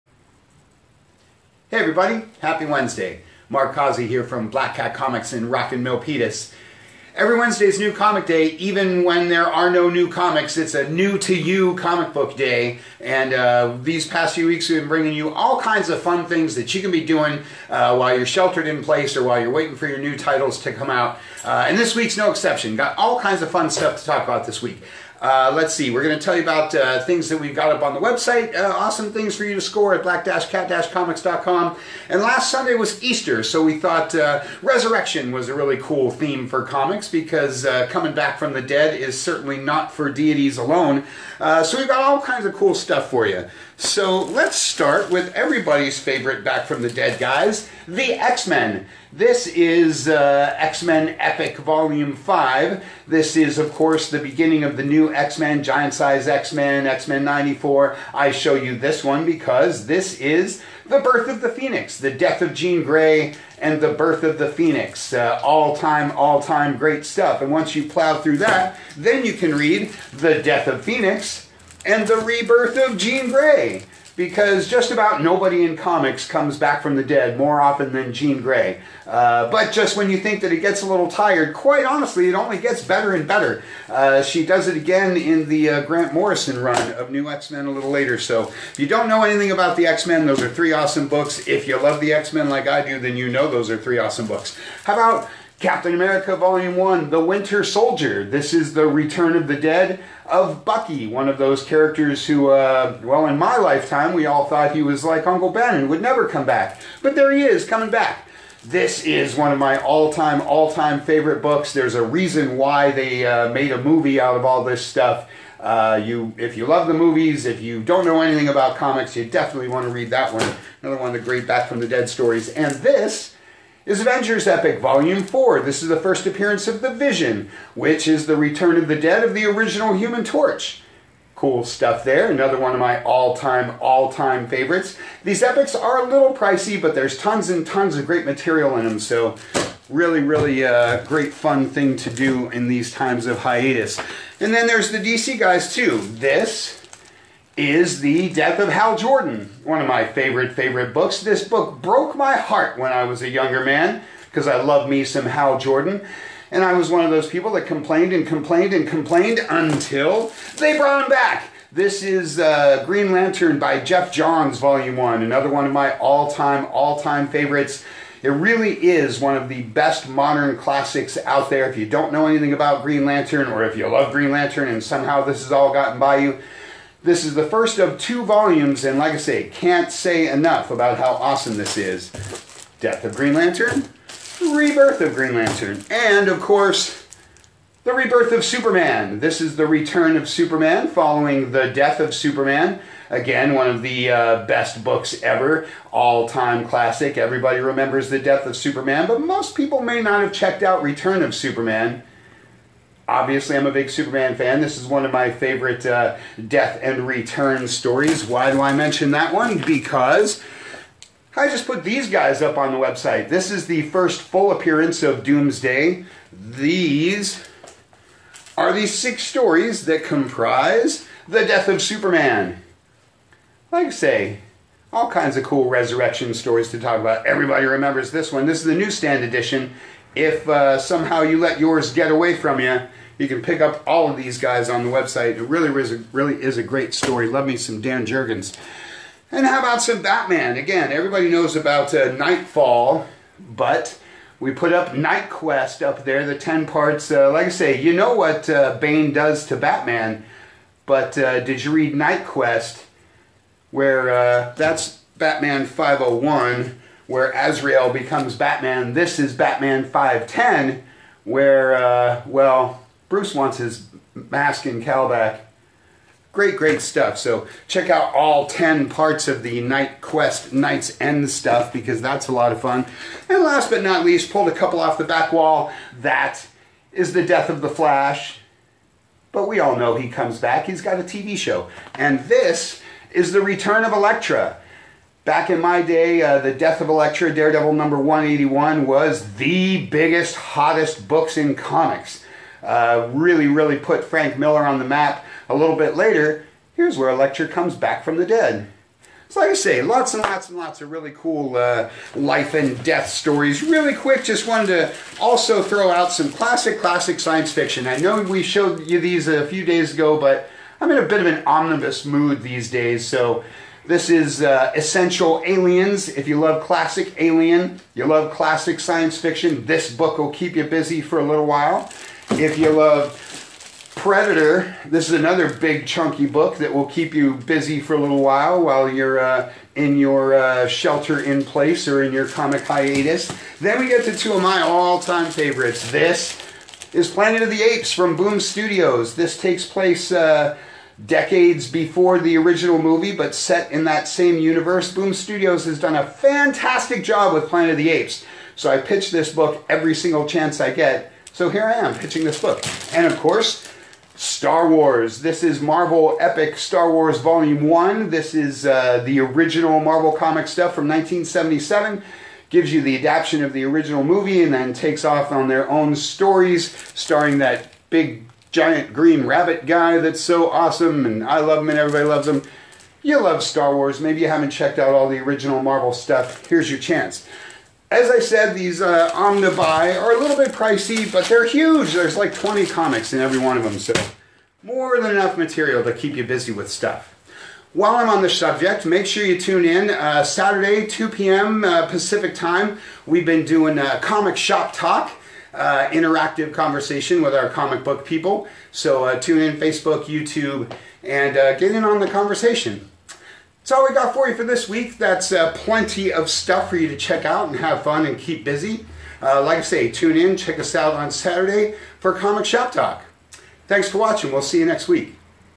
New to You Comics for the week of 4/15/20
This week we're focusing on some of the best Resurrection stories. Check out our top picks and make sure you join us on Saturday @ 2 p.m. PST where we discuss them live here on our YouTube Channel!